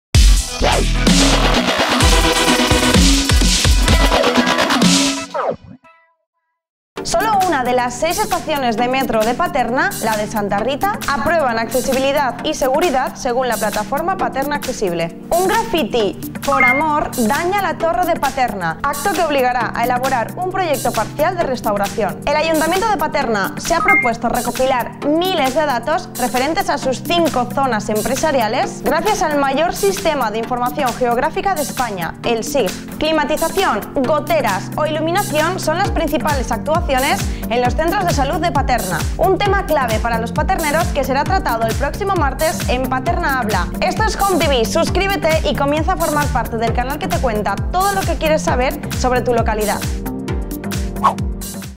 Video: Acto vandálico en la Torre de PATERNA, ¿qué pasará ahora?
Solo 1 de las 6 estaciones de Metro de Paterna, la de Santa Rita, aprueba en accesibilidad y seguridad según la plataforma Paterna Accesible.

Un grafiti “por amor”, daña la Torre de Paterna, acto que obligará a elaborar un proyecto parcial de restauración.

El Ayuntamiento de Paterna se ha propuesto recopilar miles de datos referentes a sus 5 zonas industriales, gracias al mayor sistema de información geográfica de España, el SIG.

Climatización, goteras o iluminación, primeras actuaciones en los centros de salud de Paterna, un tema clave para los paterneros que será tratado el próximo martes en Paterna Habla, no te lo pierdas.